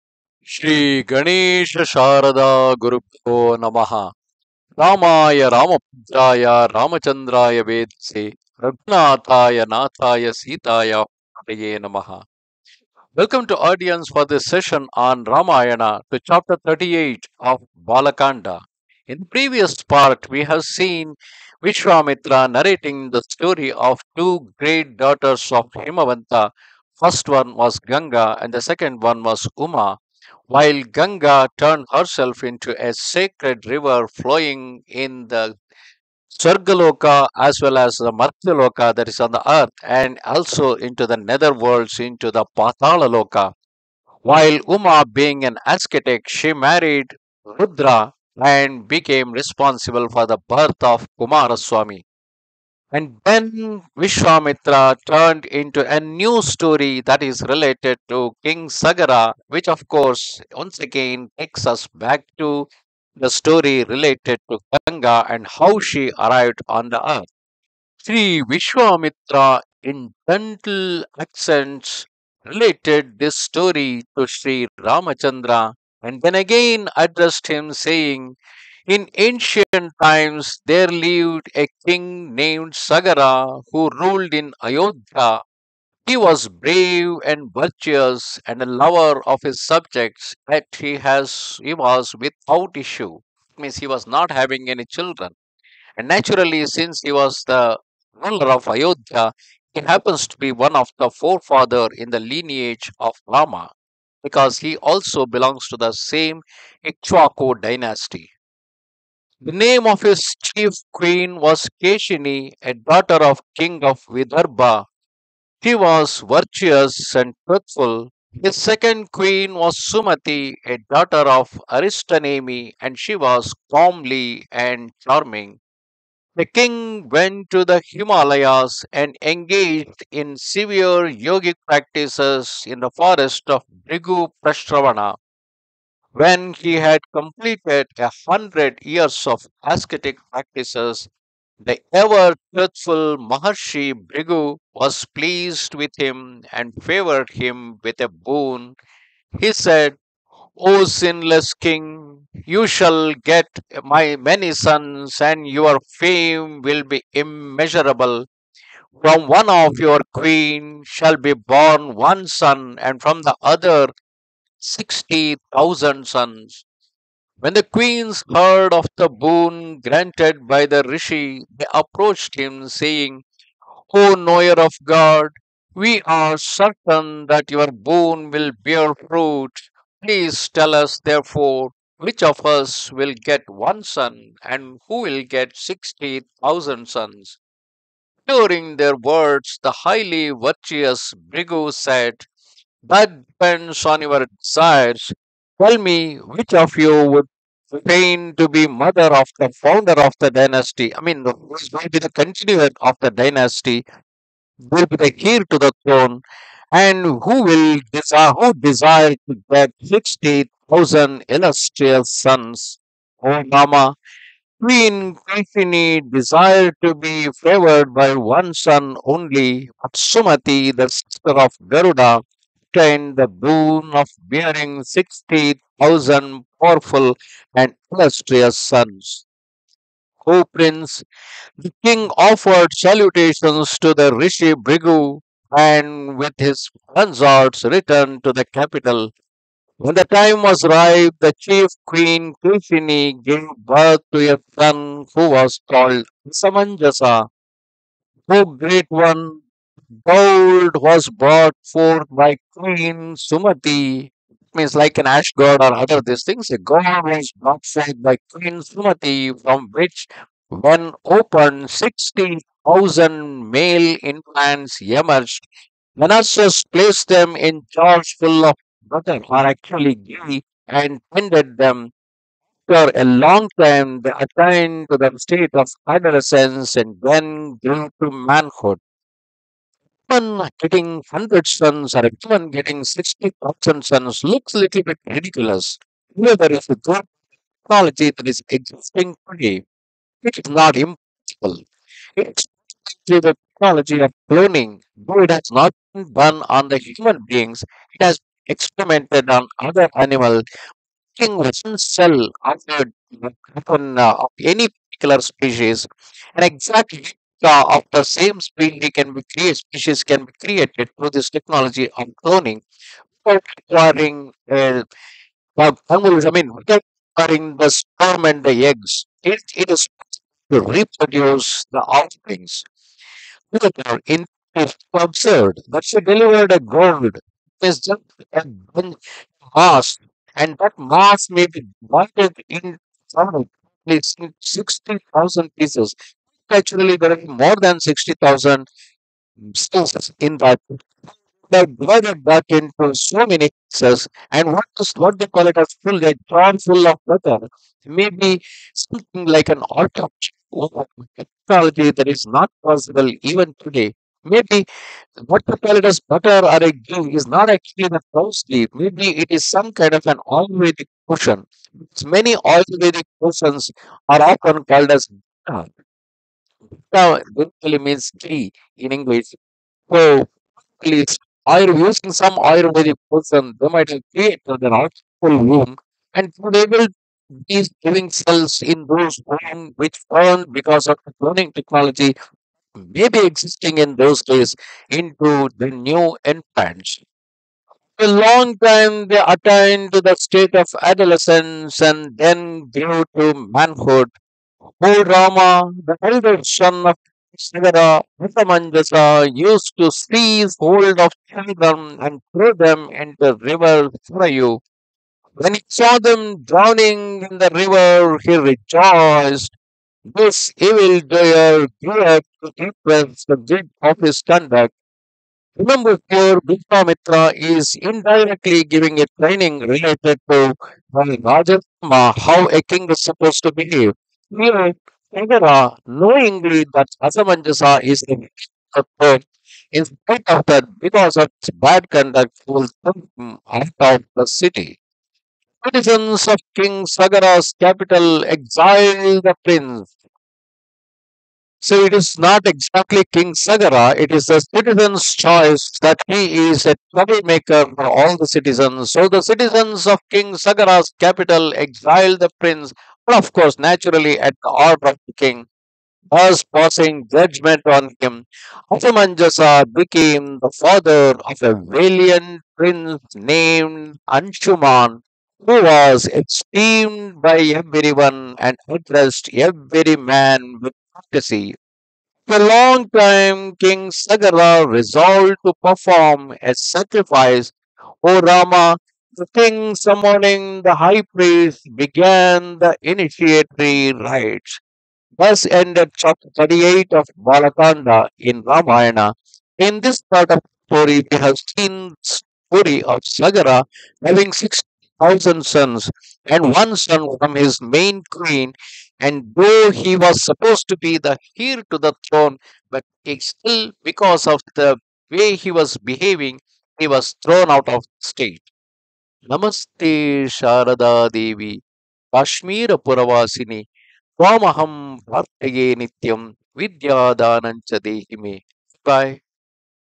Ramayana | Bala Kanda – 38 | Story of King Sagara, Sri Rama’s ancestor
Sagara having sixty thousand sons born out of divine blessings, ruled following Dharma.  His heir to throne Asamanja was sent out of Kingdom for his wrong behaviour.